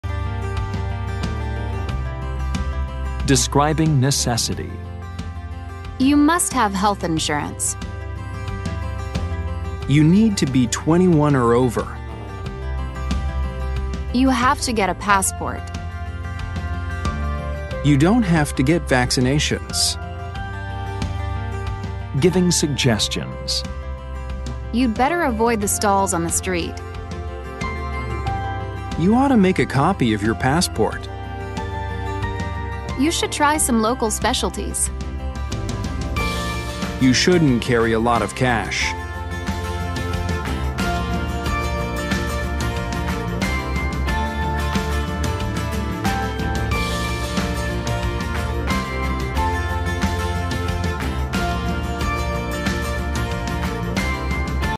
Aula de inglês: Lec Modals for necessity and suggestion
English conversation meetings to help you improve your English fluency.

RuiEnglish Lesson: Lec L02:U05:E03 Modals for necessity and suggestion

Modals for necessity and suggestion
Describing necessity
You must have a health insurance.
You need to be 21 or over.
You have to get a passport.
You don’t have to get vaccinations.

Giving suggestions
You’d better avoid the stalls on the street.
You ought to make a copy of your passport.
You should try some local specialties.
You shouldn’t carry a lot of cash.